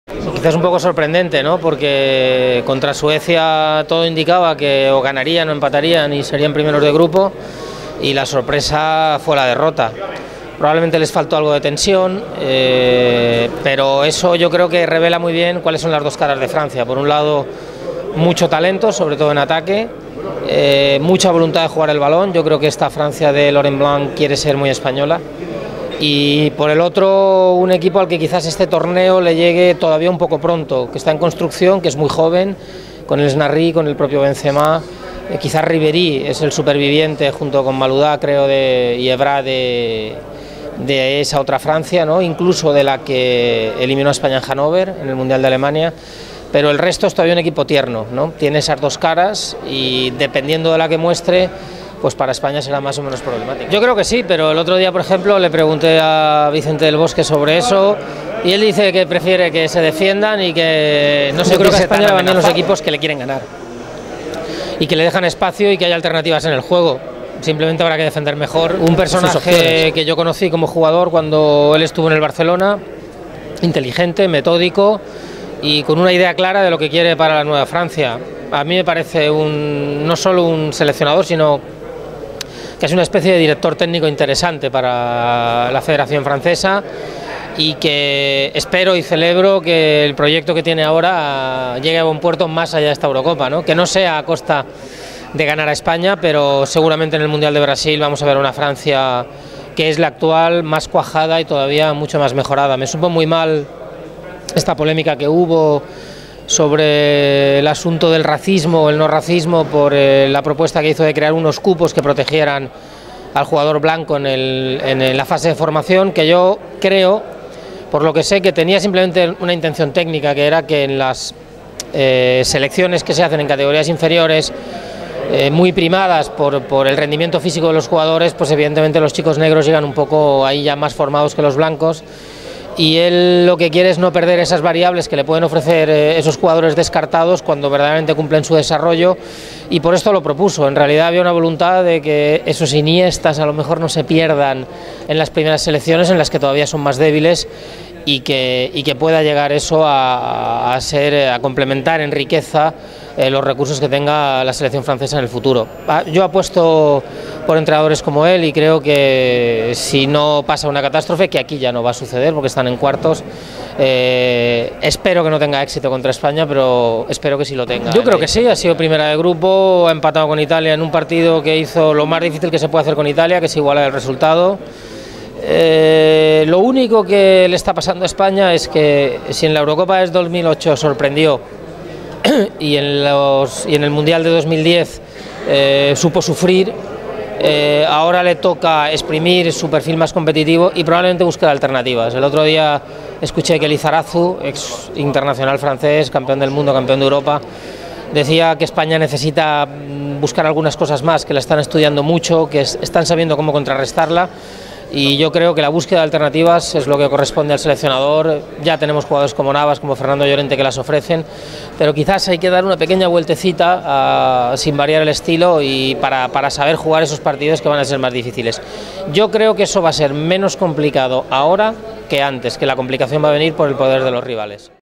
Entrevista a Orfeo Suárez periodista de El Mundo.
A dos días del partido de cuartos de final ante Francia, el periodista de El Mundo Orfeo Suárez nos da su visión acerca del encuentro, de la selección francesa y del actual estado del conjunto español.